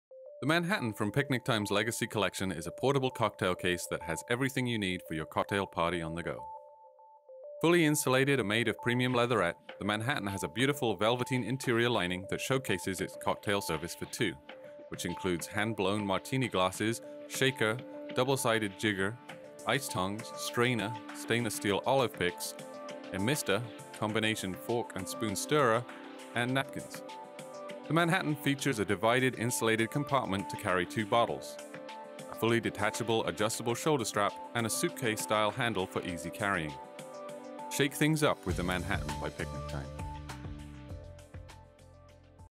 His & Hers Premium Stainless Steel Martini Bar Cocktail with Hard Case
Portable cocktail case that has everything needed for your cocktail party on the go. This His & Hers has a beautiful velveteen interior lining that helps to showcase all its amenities,. It features a divided, insulated compartment to carry two bottles, an adjustable leatherette shoulder strap and a suitcase-style handle for easy carrying. Why wait for the party, when you can bring the party with you?